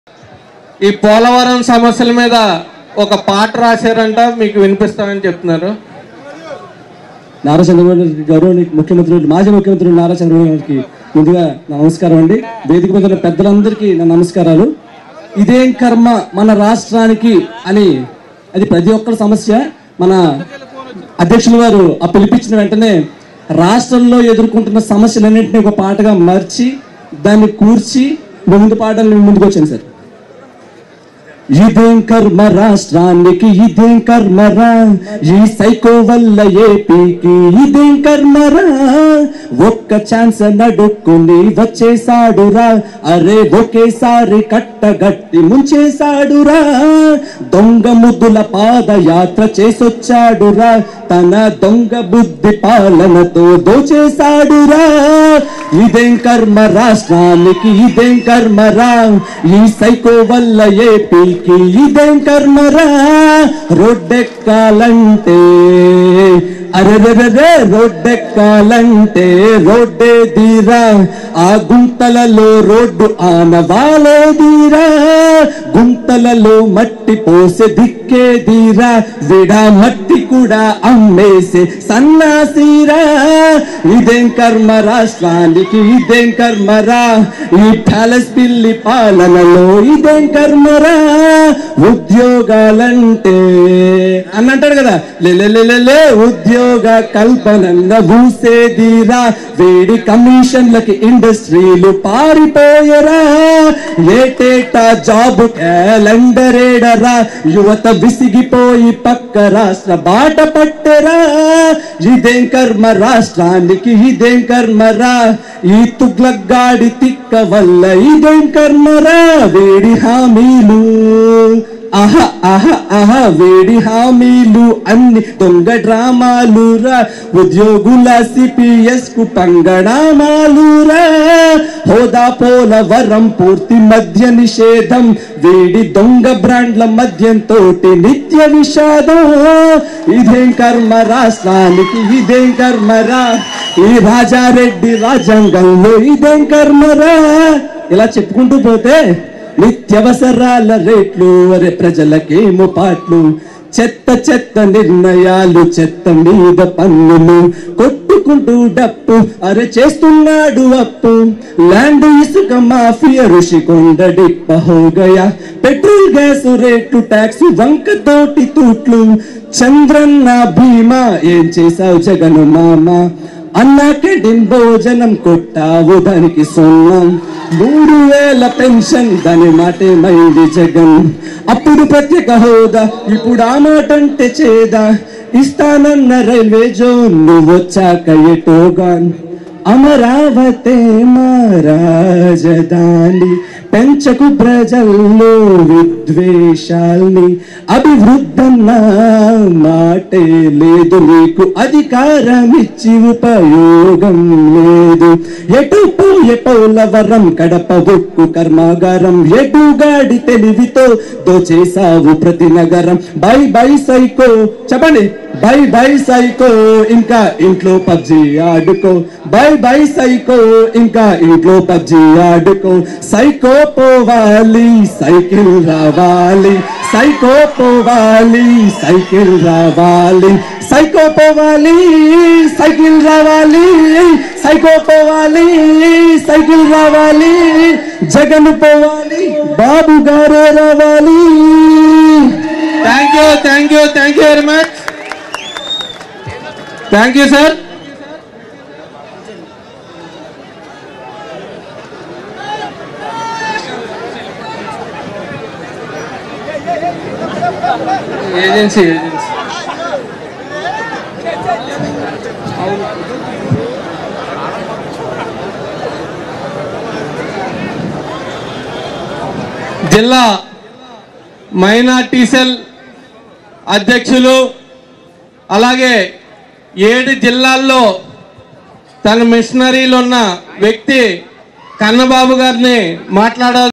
Chandrababu Naidu Funny Reaction to Satirical Song on YS Jagan | Leo News
Leo Politics

Leo Health

Leo Entertainment

Leo Devotional